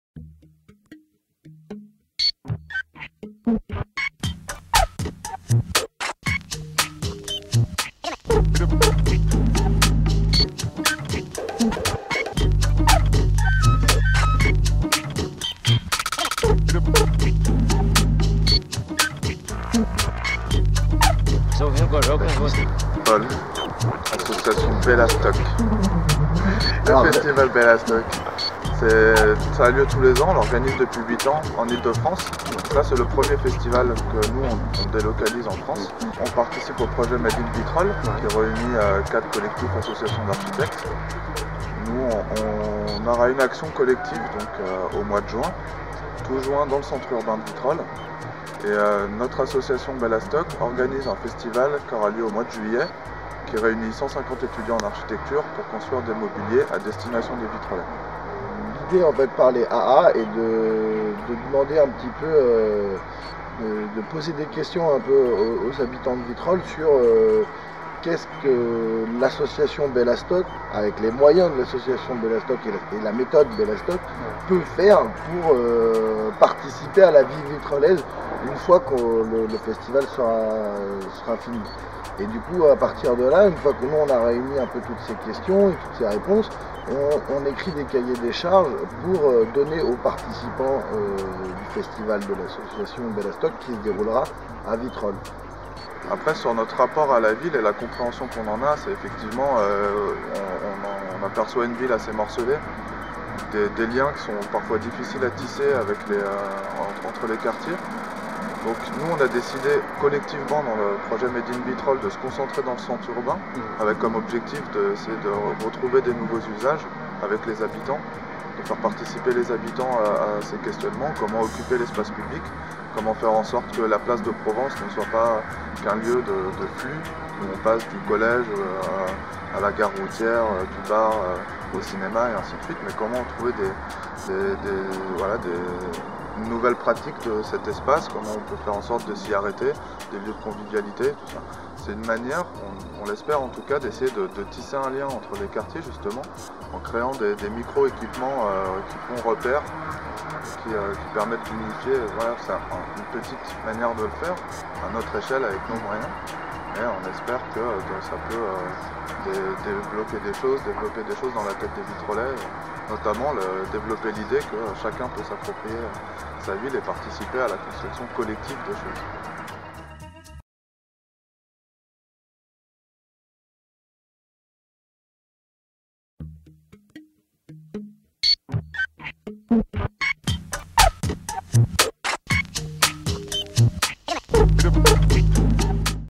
COLLECTIF BELLASTOCK - PORTRAIT
Dans le cadre de Marseille-Provence 2013 Capitale Européenne de la Culture, Bellastock est invitée par la Ville de Vitrolles à participer à Vitrolles Echangeur, un véritable projet culturel pour la ville.

La mission conduite par Bellastock a pour objectif la construction d'installations urbaines au coeur de la ville, une performance créative et constructive unique en son genre.

Trois autres collectifs prennent part au projet « Made in Vitrolles » : ETC (Strasbourg), les Saprophytes (Lille) et EXYZT (Paris/Londres).

Juin 2013 : construction et activation des installations architecturales, des animations festives et des ateliers de fabrication de mobilier et de peinture au sol par les quatre groupes d'architectes en partenariat avec les associations locales.

18-21 juillet 2013 : le festival Bellastock « Made in Vitrolles » réunit 150 étudiants dans un chantier au cœur de la ville. L'objectif est de profiter de notre énergie collective pour nourrir le projet imaginé pour Vitrolles Echangeur.